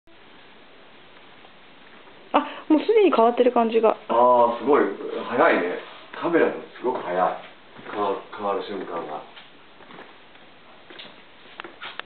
Japanese trick art
Japanese trick art (a kind of "trompe-l'œil") in a painting of Bishamon-do Temple (1623), Kyoto, Japan.